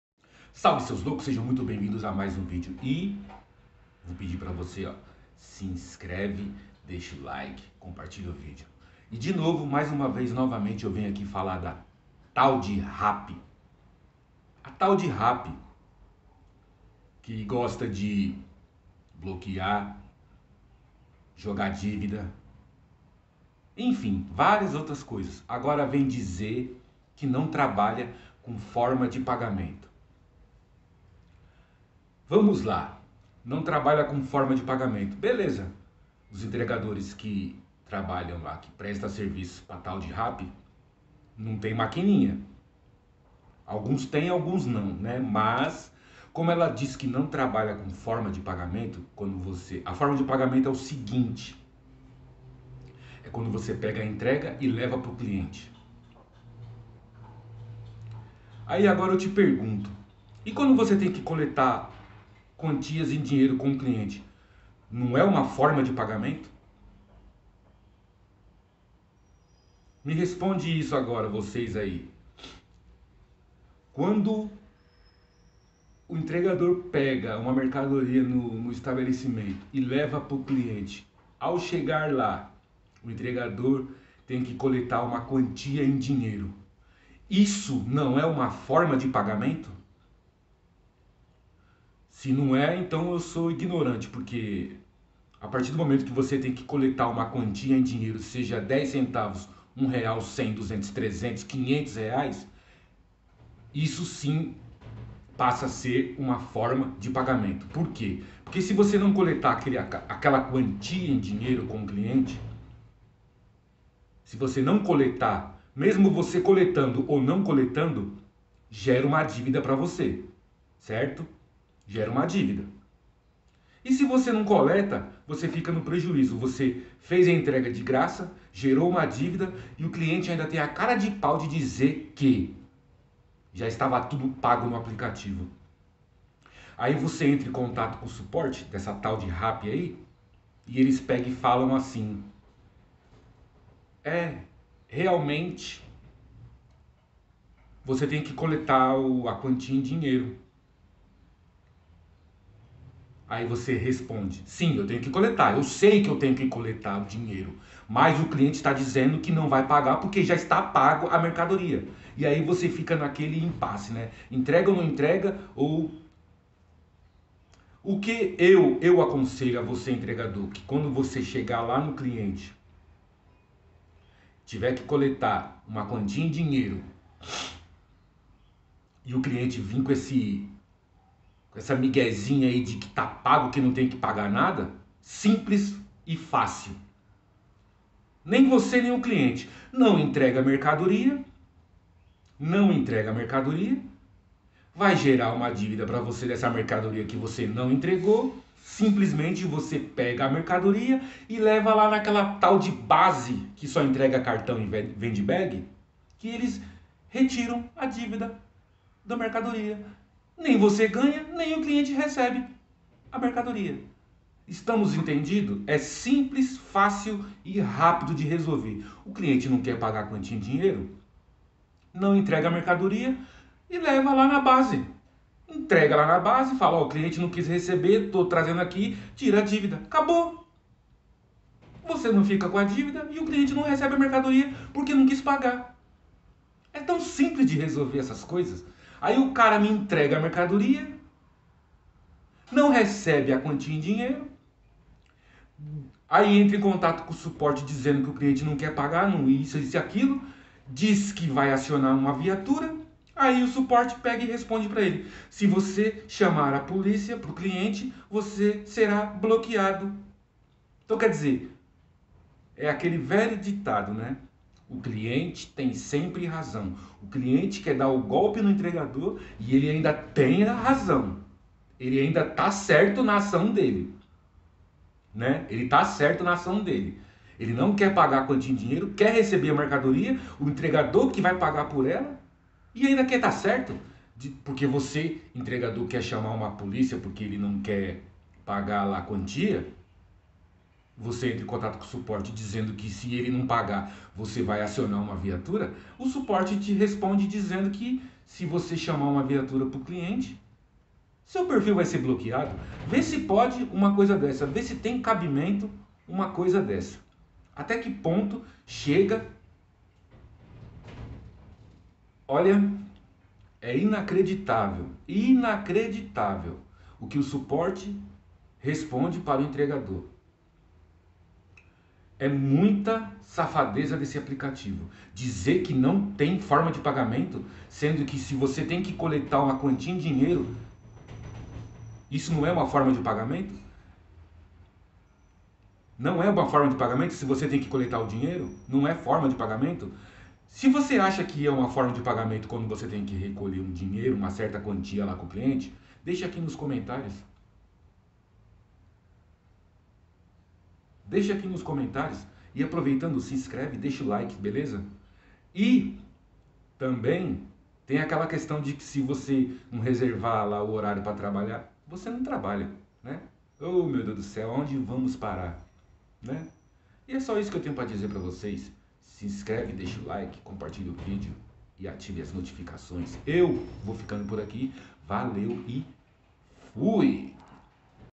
RAPPI COLETAR DINHEIRO É SIM UMA FORMA DE PAGAMENTO
RAPPI DIZ QUE NÃO TRABALHA COM FORMA DE PAGAMENTO E QUANDO TEM QUE COLETAR DINHEIRO O QUE SERIA